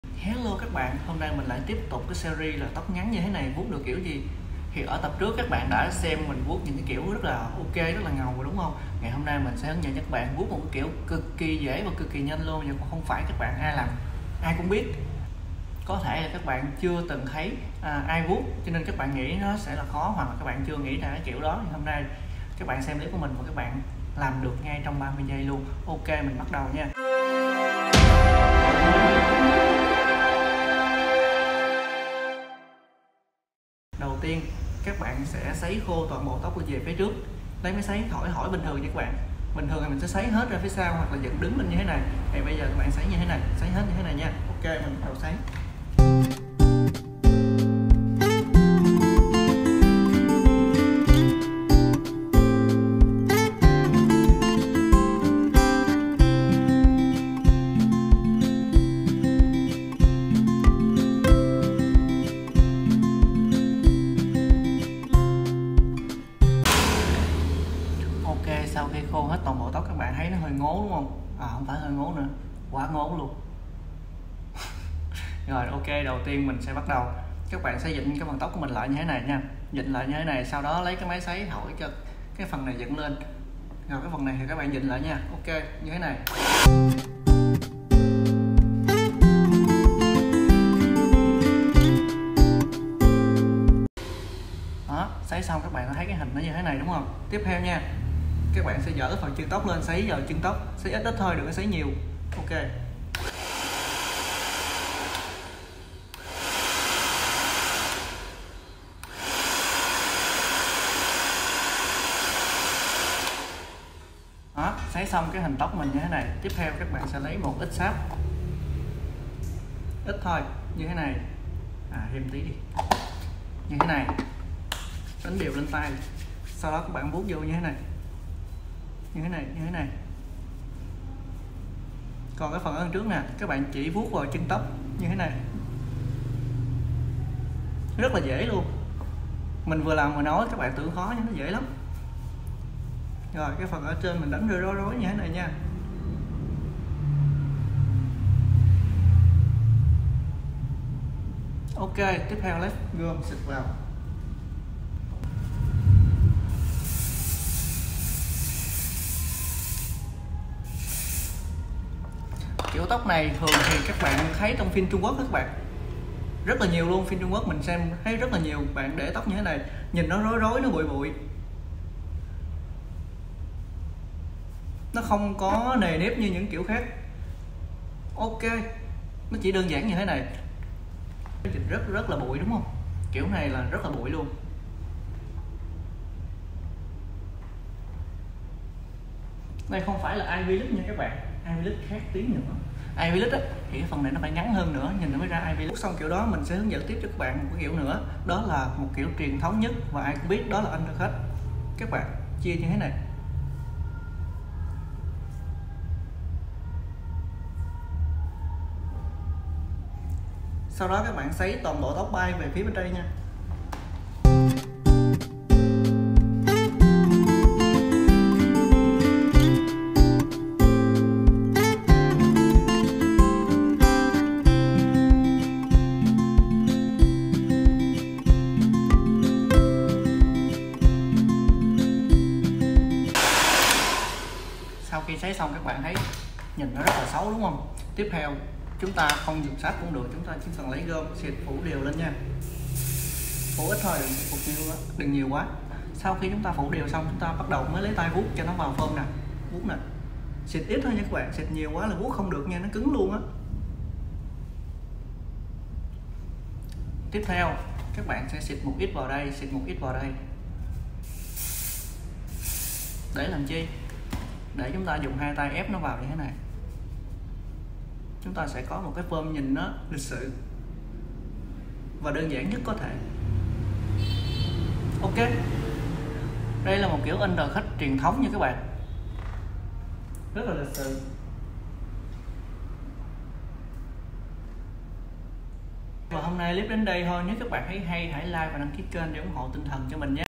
Cách Vuốt Tóc Nam Ngắn 7/3 Undercut Đơn Giãn | Ai Cũng Làm Được Không Cần Đi Uốn Tóc
Mua gôm sáp chính hãng giá rẻ tại